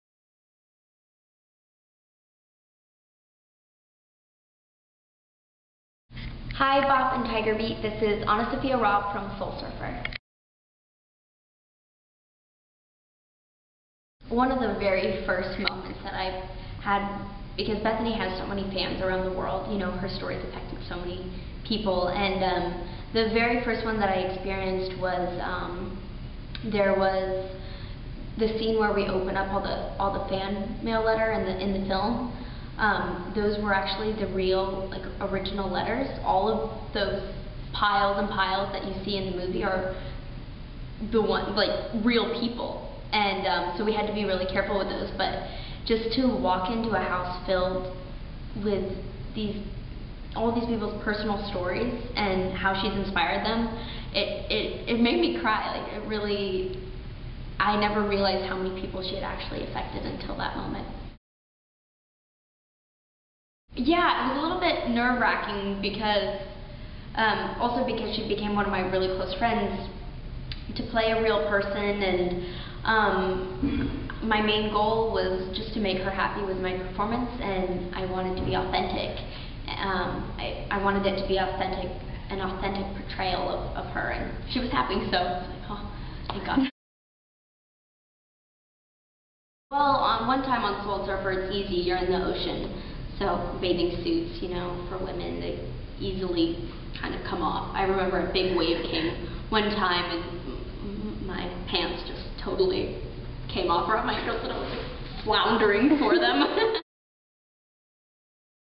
EXCLUSIVE! 3 Questions With Soul Surfer Star, AnnaSophia Robb! (BOP & Tiger Beat)
BOP and Tiger Beat got to sit down with AnnaSophia Robb, who stars in Soul Surfer! She answered three juicy questions about the movie!